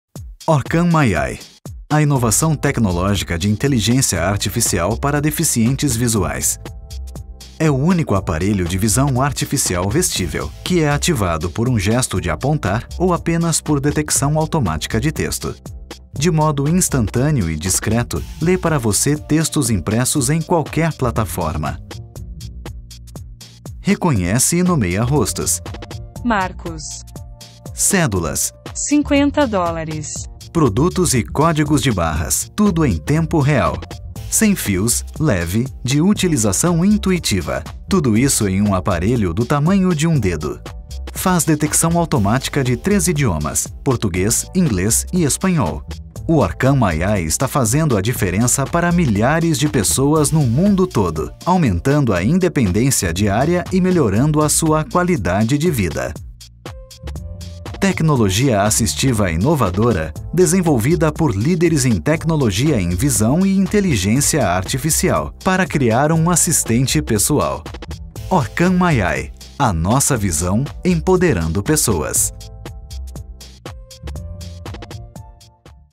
OrCam MyEye 2 - Principais Características (BR)
Com o OrCam MyEye, uma pequena câmera inteligente conectável magneticamente a qualquer armação de óculos, pode finalmente apreciar o jornal da manhã, ler qualquer livro e saber o que há no menu.
Reconheça os seus entes queridos, faça compras sem auxílio e viva uma vida mais independente! Junte-se a dezenas de milhares de pessoas cegas e com deficiência visual em todo o mundo que mudaram suas vidas com a OrCam.